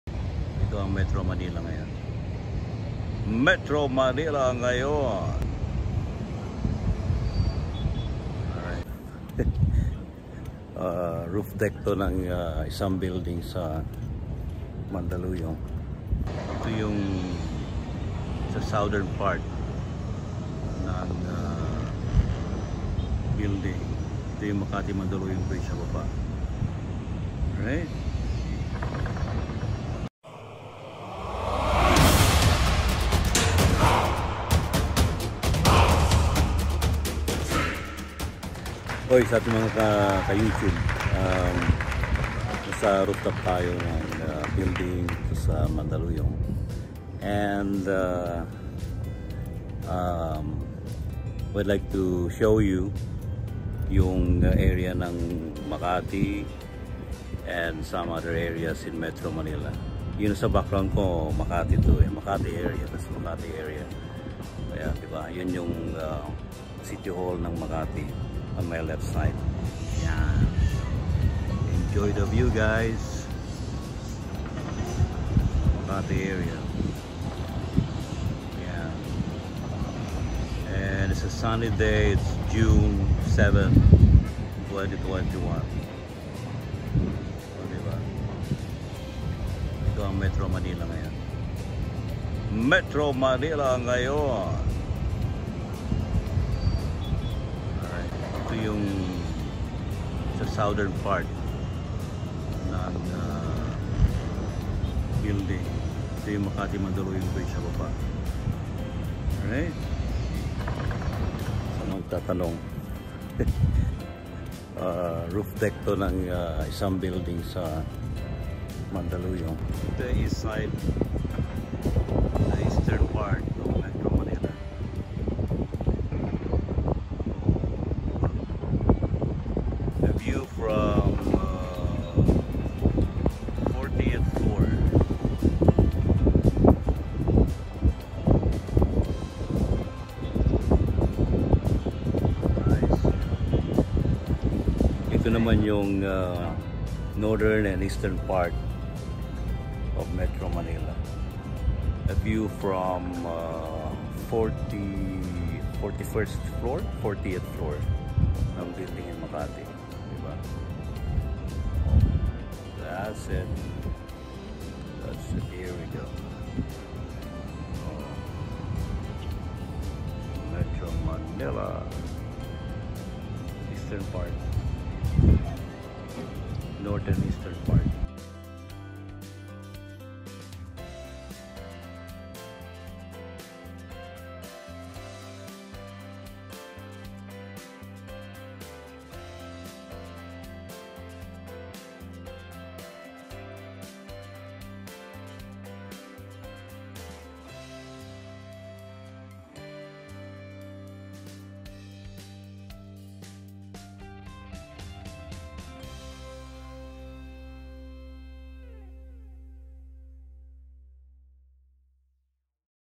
Metro Manila Today (Bird’s Eye View)
WATCH! This is Metro Manila today, a view from a high-rise building roof deck.
I put in a video file footage taken last 2018 to compare with the present scenery.
Watch out for more video footages of Metro Manila in the future.